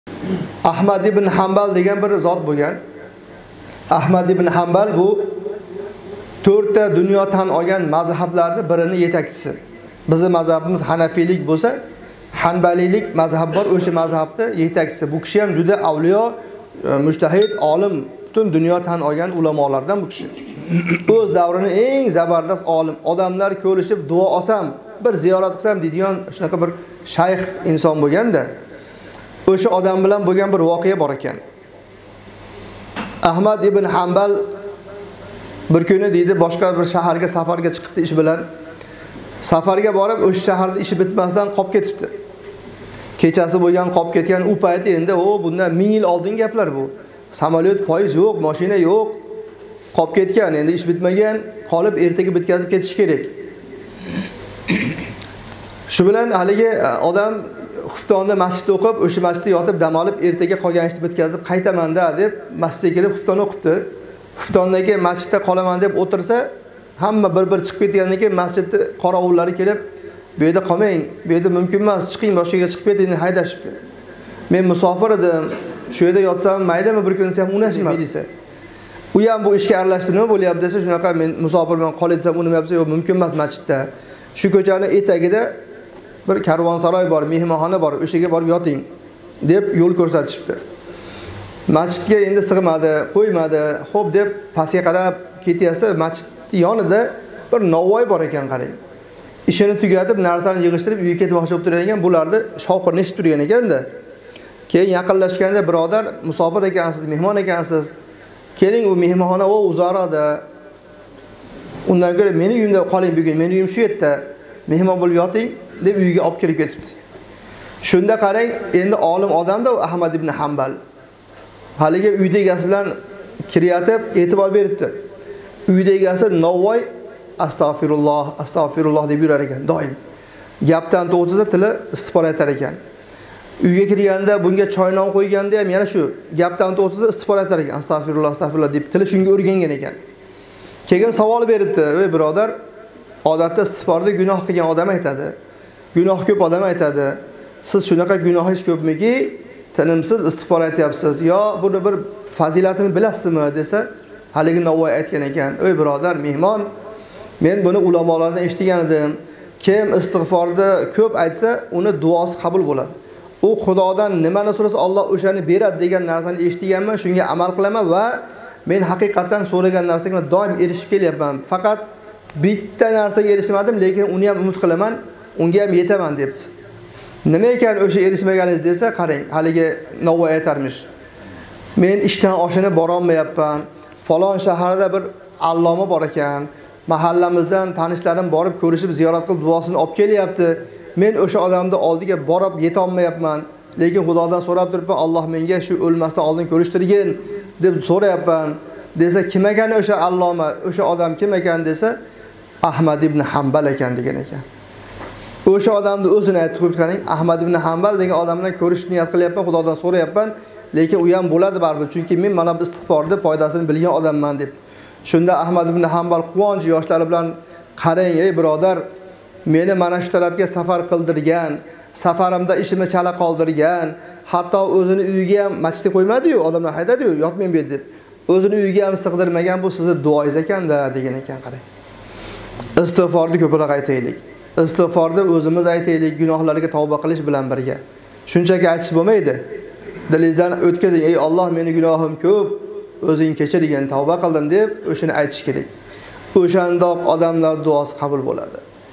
ИСТИҒФОРНИНГ ФАЗИЛАТЛАРИ (АХМАД ИБН ХАНБАЛ)
Булокбоши тумани КИРГИЗБОЙ МАСЖИДИ имом хатиби Миркомил домла Ботиров/ Buloqboshi tumani Qirg'izboy masjidi imom hatibi Mirkomil domla Botirov